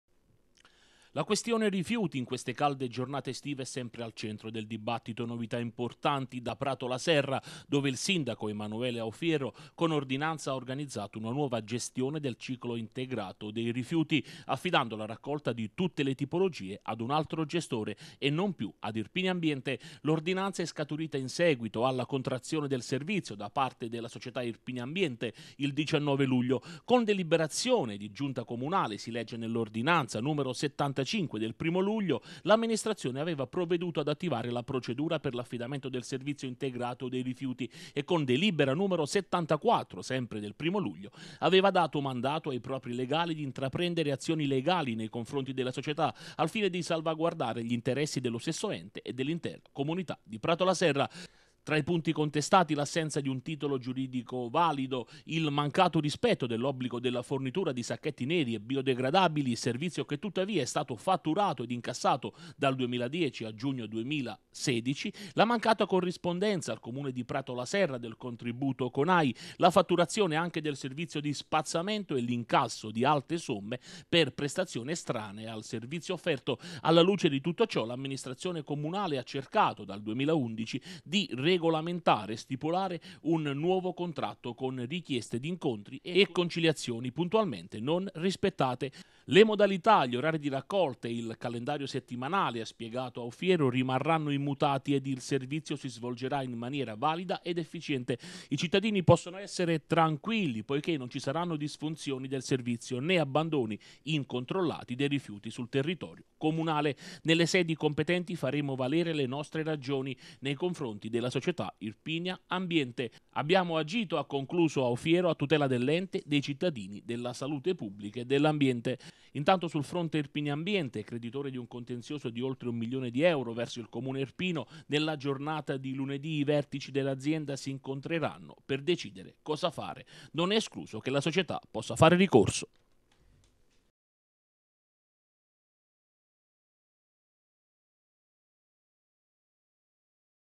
Rifiuti Pratola Serra, affidato servizio ad altro gestore. Irpiniambiente valuta l'ipotesi ricorso
Nel pomeriggio di ieri, attraverso un comunicato stampa, il sindaco Emanuele Aufiero ha reso noto che il servizio di raccolta rifiuti è stato affidato a un altro gestore. Intanto Irpiniambiente deciderà lunedì se fare o meno ricorso.